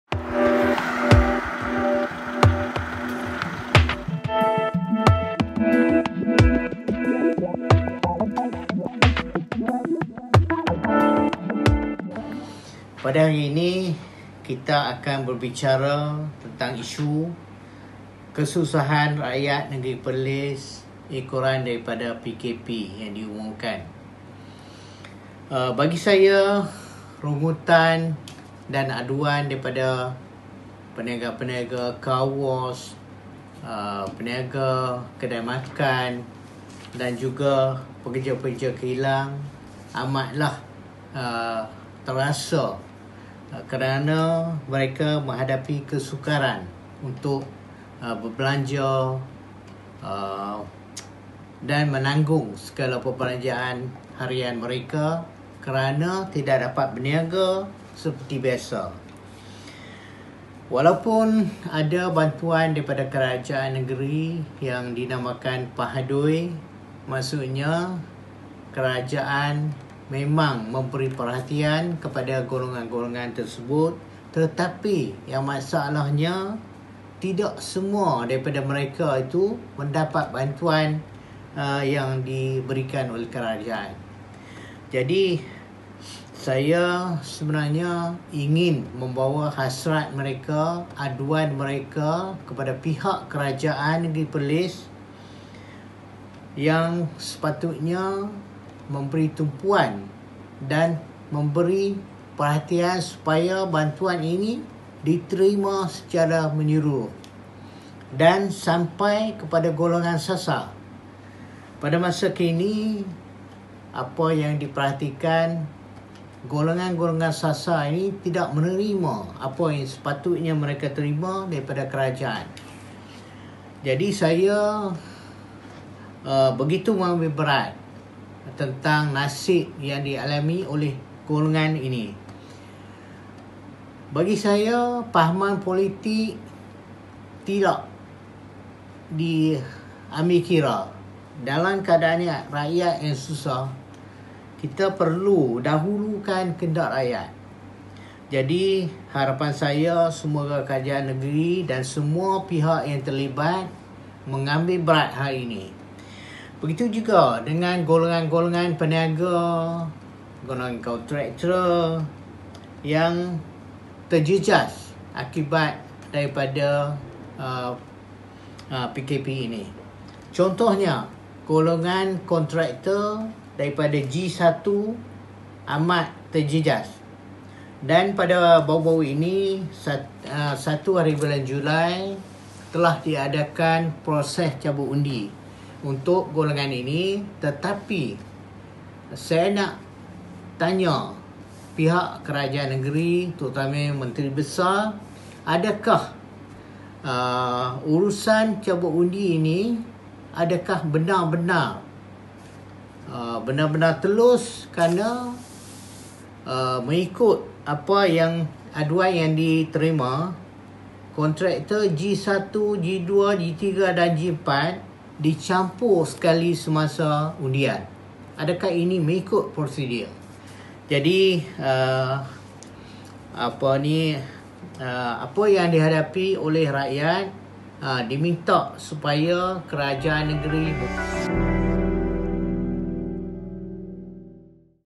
Peniaga terkesan, bantuan tak sampai kerana pilih kasih dakwa Putra Perlis
Parti Bumiputera Perkasa Malaysia (PUTRA) meminta kerajaan memberi perhatian kepada golongan terjejas terutamanya peniaga dan usahawan di Perlis.

Menurut Naib Pengerusi Putra Perlis, Shaharuddin Ismail sejak pelaksanaan PKP 12 Mei lalu, ramai pengusaha perniaagan sendiri terjejas teruk dan tiada sumber pendapatan.

Shaharuddin turut berkata walaupun Perlis telah memperkenalkan ‘Program Pahadoi (Ambil Peduli)’ untuk rakyat yang terjejas namun ia tidak sampai ke golongan B40 dan M40.

"Program ini hanya simbolik untuk diberikan kepada ahli-ahli yang menyokong kerajaan negeri sahaja," katanya.